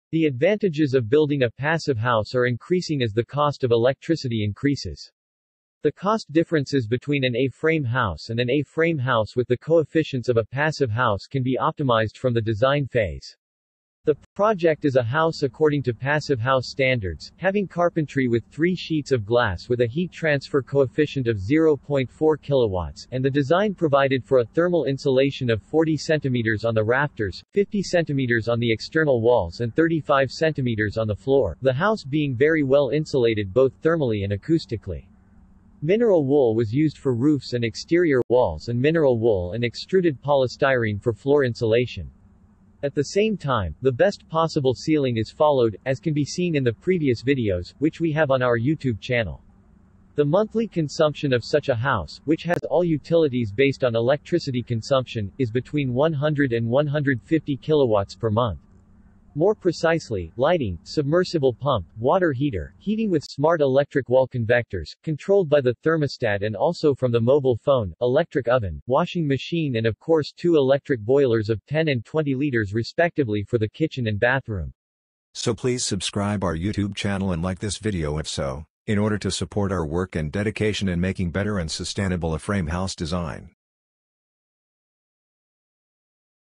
How expensive is to build a passive house | A-frame
The advantages of building a passive house are increasing as the cost of electricity increases. The cost differences between an A-frame house and an A-frame house with the coefficients of a passive house can be optimized from the design phase.